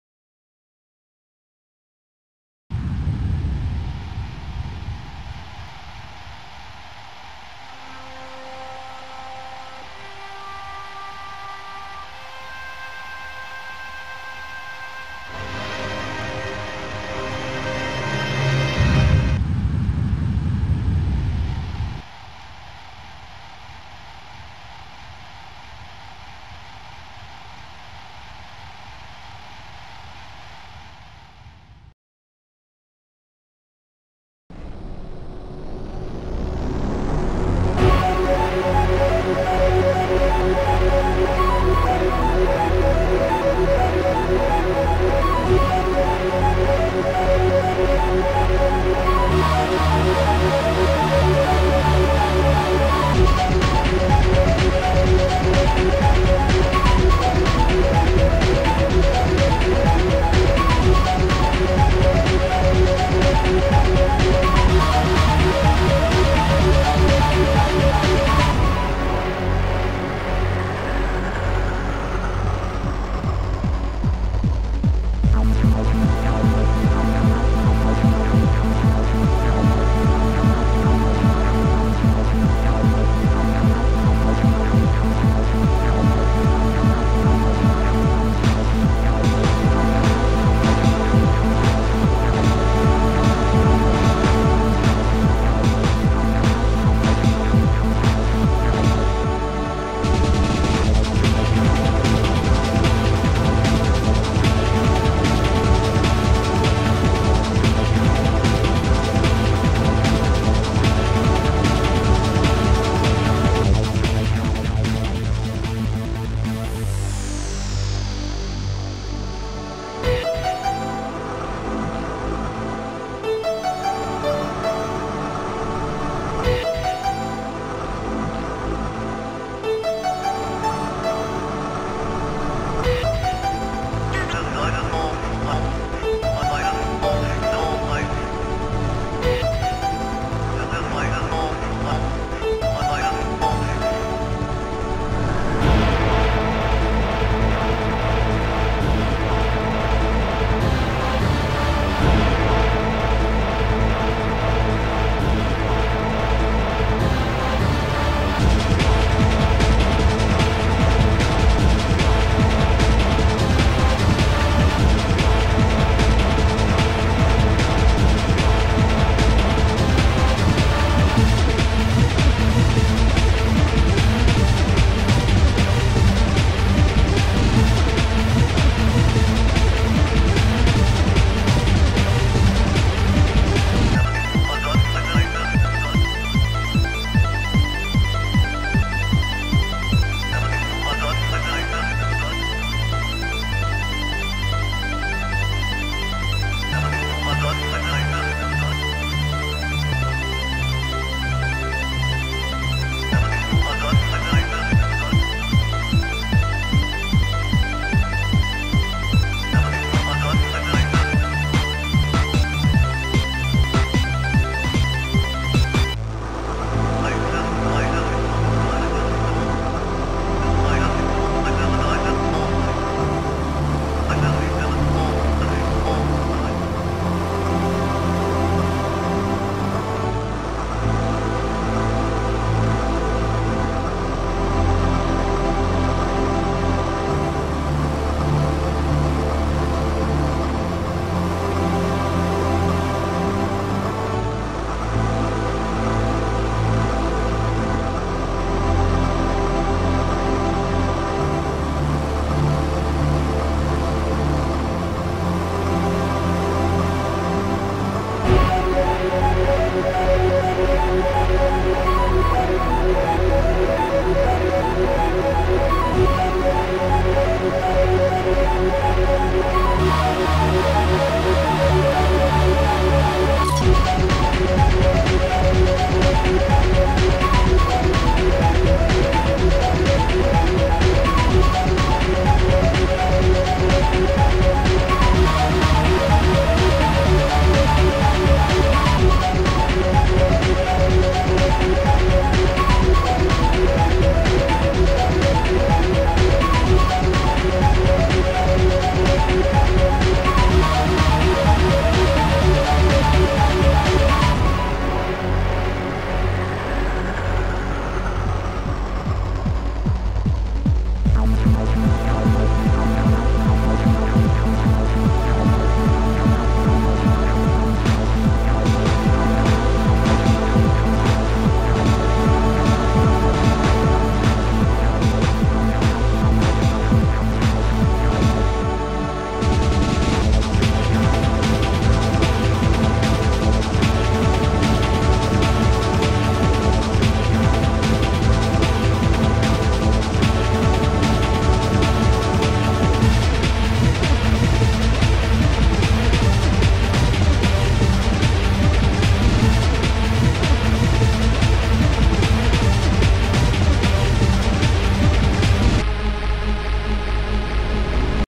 Amiga Trackmo : Negative FX / Reality (2014)
(re-up. fixed dots parts)
oldskool competition A500. 2nd @ Datastorm 2014
prod.php?which=62564

great music by Pete